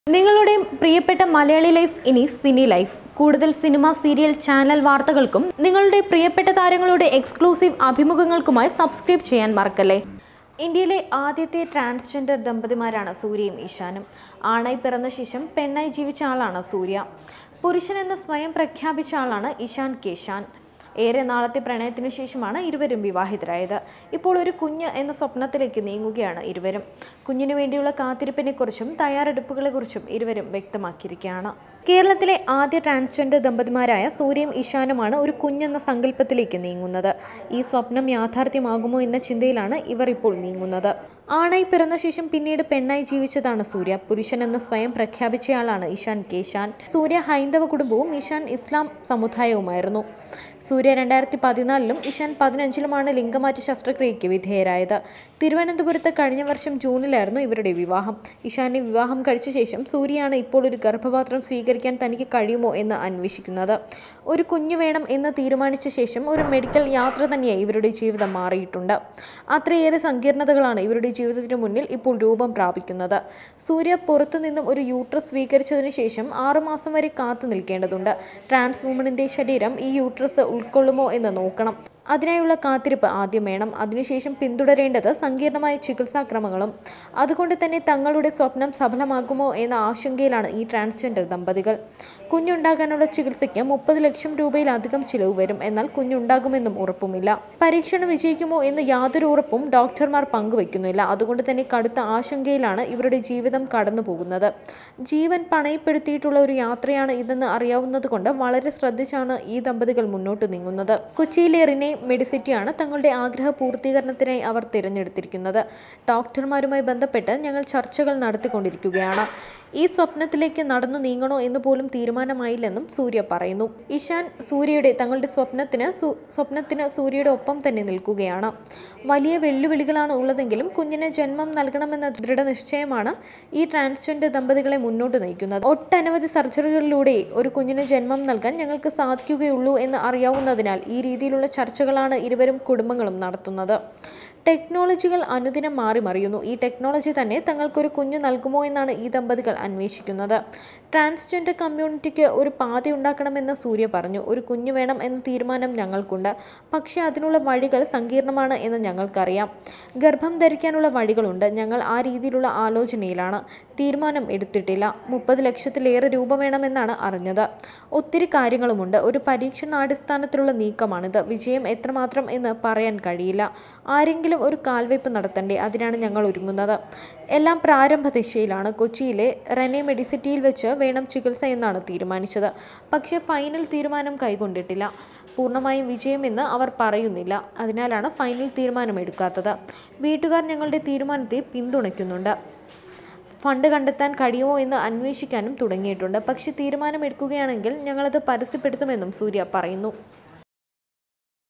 ചരിത്രം വഴിമാറുമോ? സൂര്യയും ഇഷാനും കണ്‍മണിക്കായി കാത്തിരിക്കുന്നു..!
ഇന്ത്യയിലെ ആദ്യത്തെ ട്രാന്‍സ് ജെന്‍ഡര്‍ ദമ്പതിമാരാണ് സൂര്യയും ഇഷാനും. ആണായി പിറന്നശേഷം പെണ്ണായി ജീവിച്ച ആളാണ് സൂര്യ. പുരുഷനെന്ന് സ്വയം പ്രഖ്യാപിച്ച ആളാണ് ഇഷാന്‍ കെ ഷാന്‍. ഏറെ നാളത്തെ പ്രണയത്തിന് ശേഷമാണ് ഇരുവരും വിവാഹിതരായത്. ഇപ്പോള്‍ ഒരു കുഞ്ഞ് എന്ന സ്വപ്‌നത്തിലേക്ക് നീങ്ങുകയാണ് ഇരുവരും. കുഞ്ഞിനു വേണ്ടിയുളള കാത്തിരിപ്പിനെക്കുറിച്ചും തയ്യാറെടുപ്പിനെക്കുറിച്ചും ഇരുവരും വ്യക്തമാക്കിയിരിക്കയാണ്.